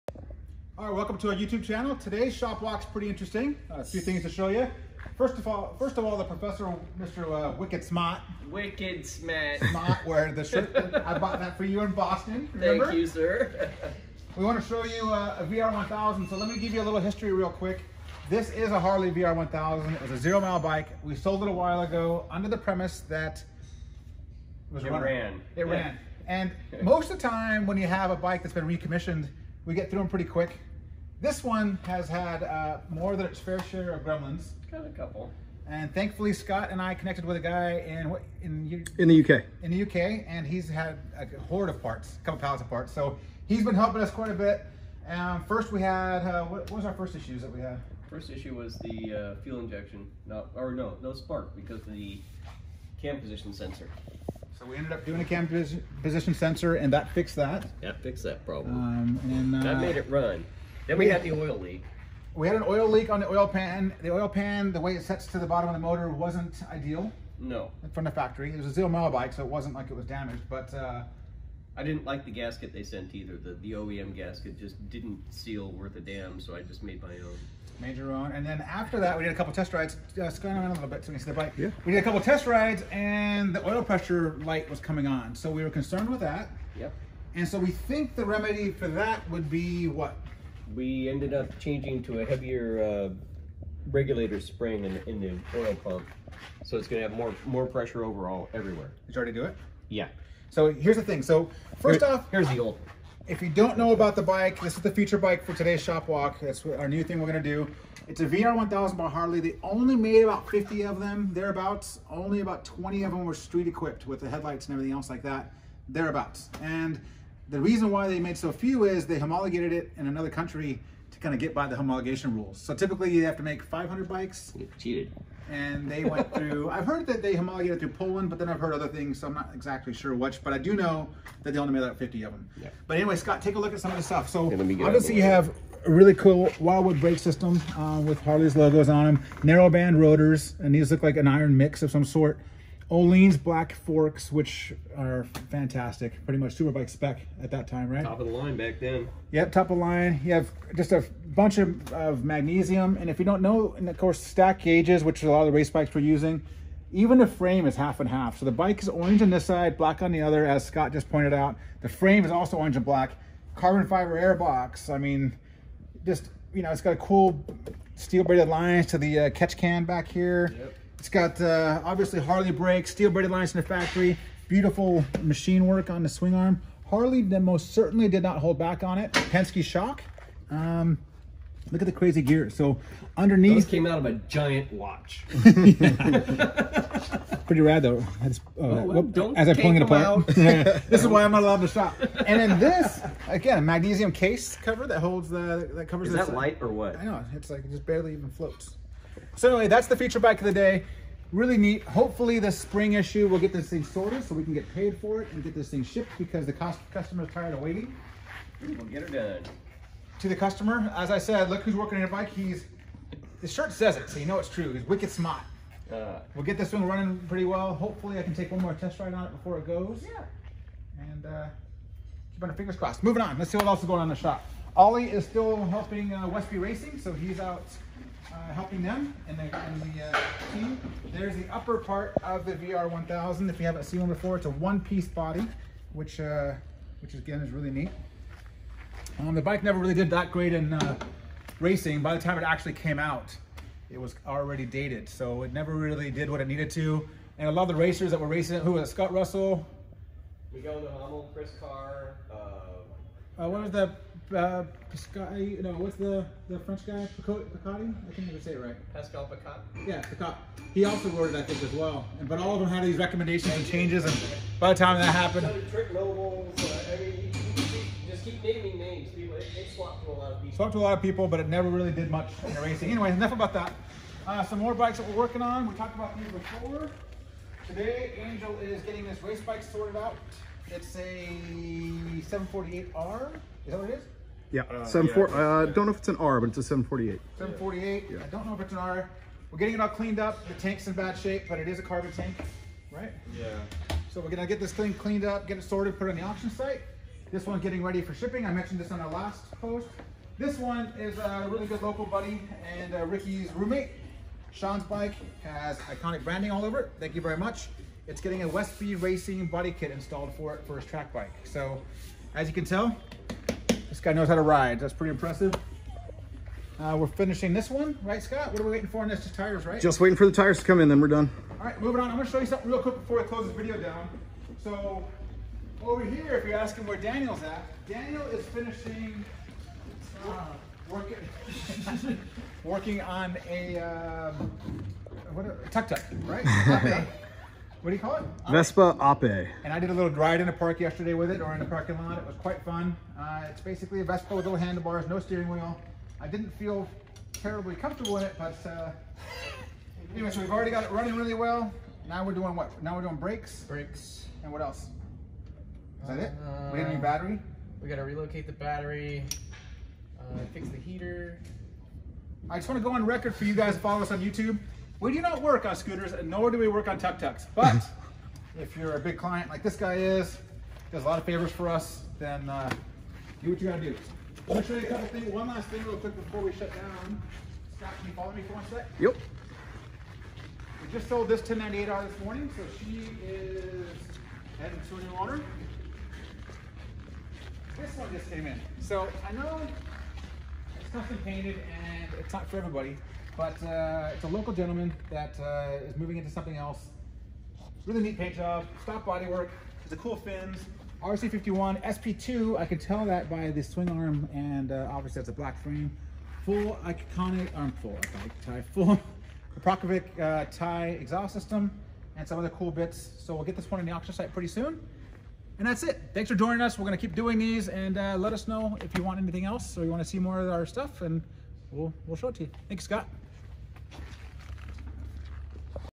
The Shop Walk - 5/21 ft. Harley-Davidson VR1000
On today's Shop Walk, Adam talks you through a 0-mile Harley-Davidson VR1000 that we're making a runner.

We've also got a Ducati 748 track bike, Yamaha R1, Piaggio Ape, and a Honda RC51!